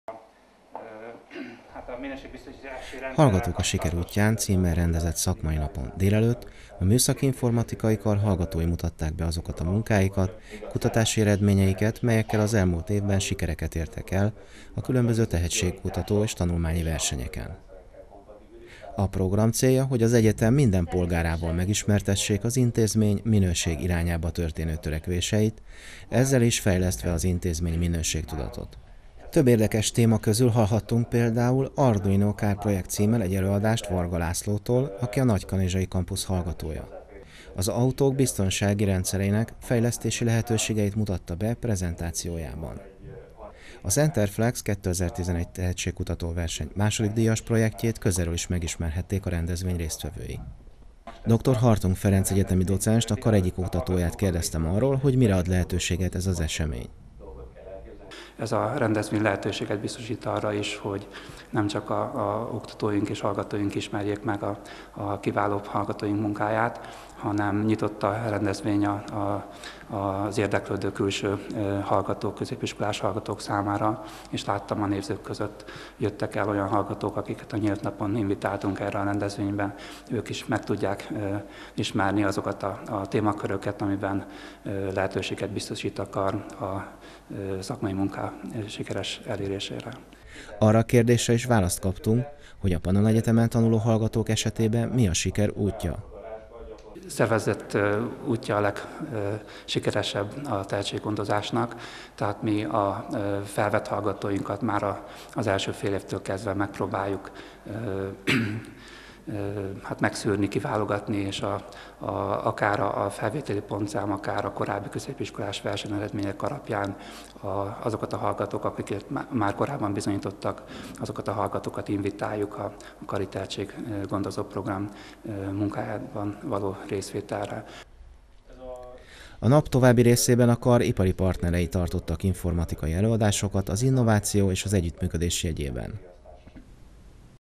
Pannon Egyetem Napja MIK
A Pannon Egyetem Napját 2012. február 2-án rendezték Veszprémben és Keszthelyen. A rendezvényre a karok különböző szakmai programokkal és előadásokkal készültek.